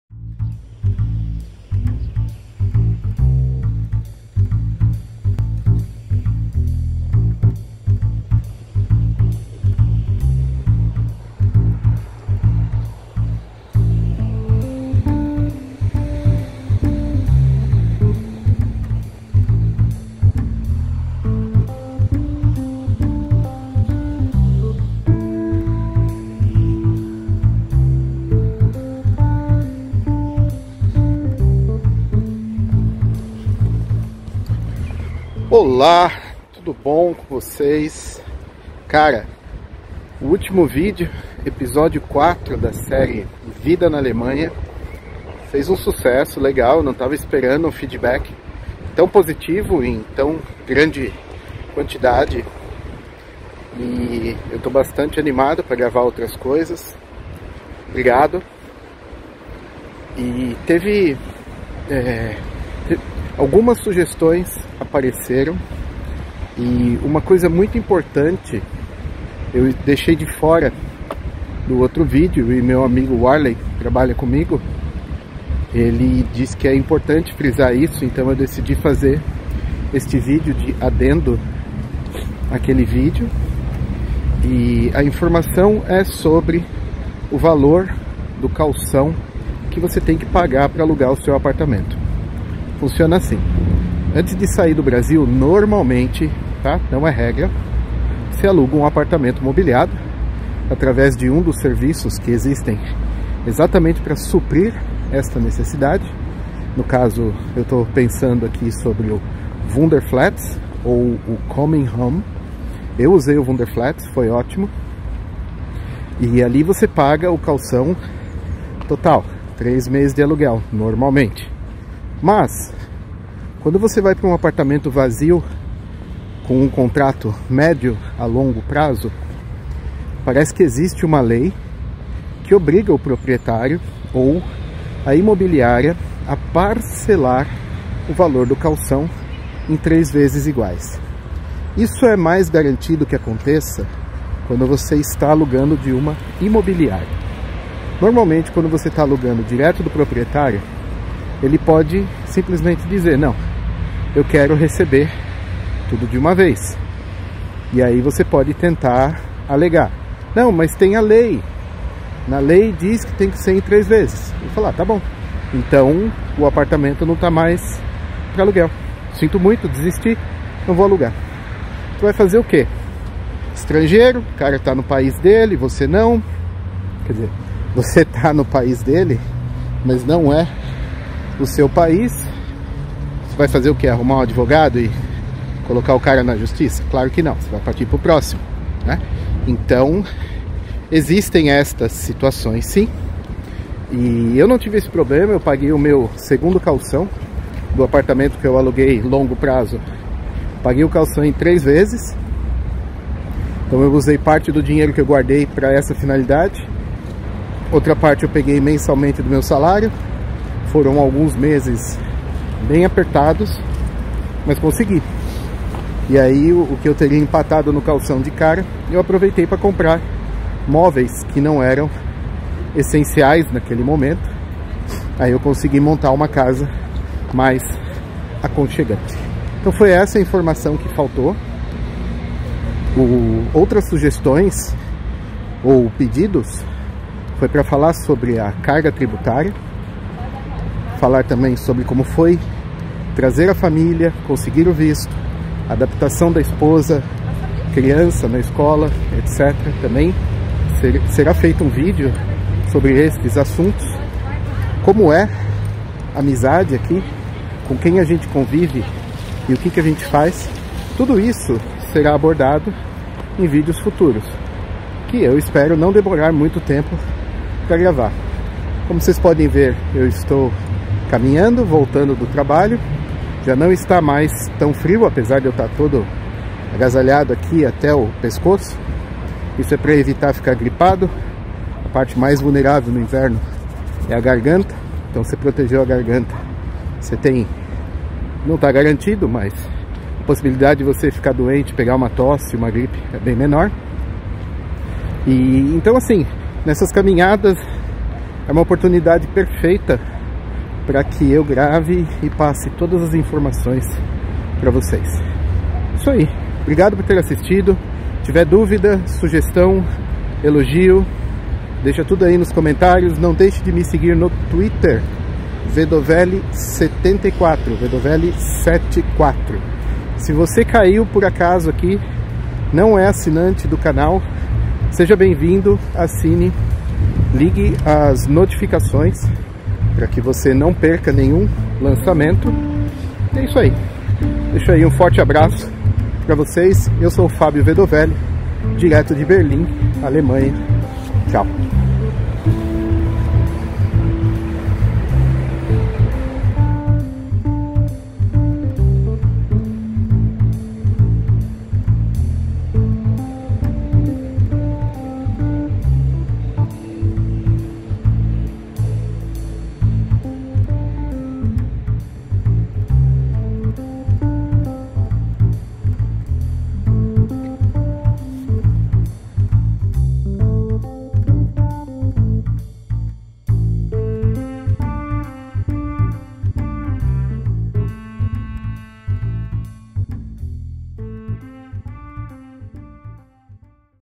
Vida na Alemanha, adendo ao episódio 4 - Depósito caução para aluguel de imóveis
No episódio 4 eu falei sobre os desafios de quem está para se mudar para a Alemanha, mais especificamente Berlin. Um dos maiores desafios é o de encontrar moradia. Meu amigo Warley apontou que a informação que passei ficou incompleta e este vídeo visa complementa-la.